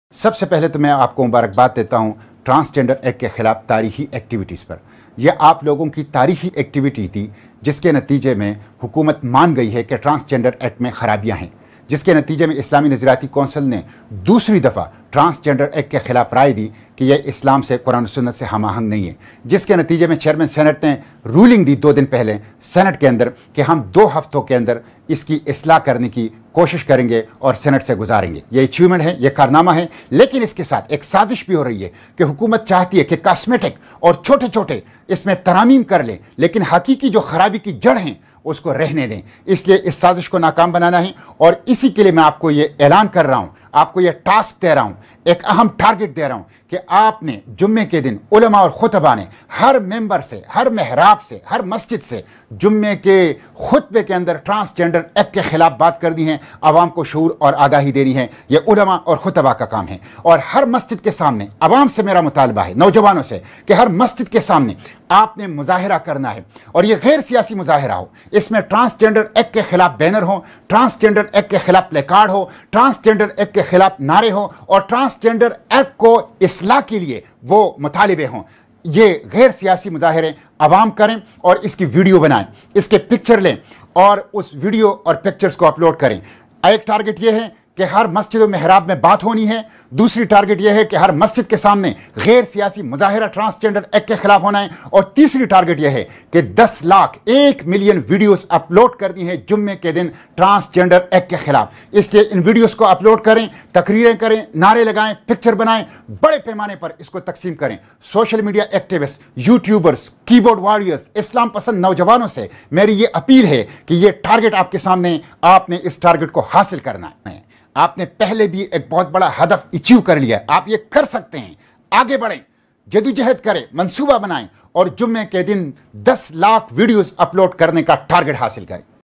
Transgender Act 2022, Jamat - e - Islami calls for protest | IDN
Jamaat e Islami leader Senator Mushtaq Ahmed Khan has appealed to the nation to Protest on Friday against transgender act which has approved by the Parliament  .
Please follow us on:
Ittehad Digital Network